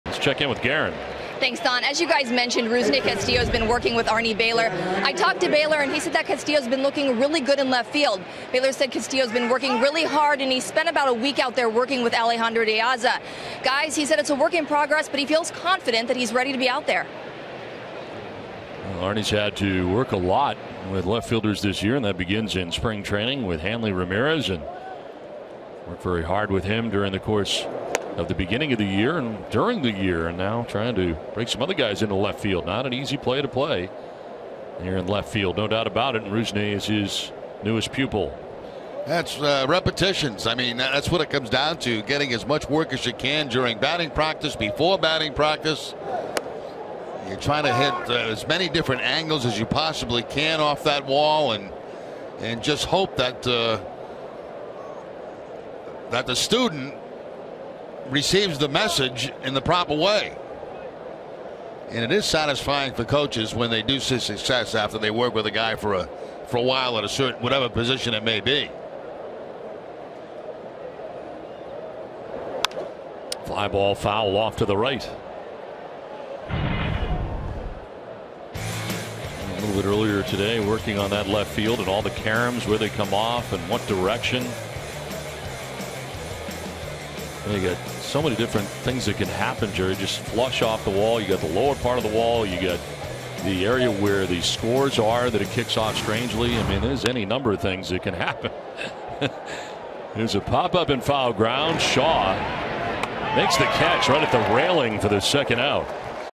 NYY@BOS: Castillo works out in left field
9/2/15: Rusney Castillo works out in left field at Fenway Park as he transitions into the new position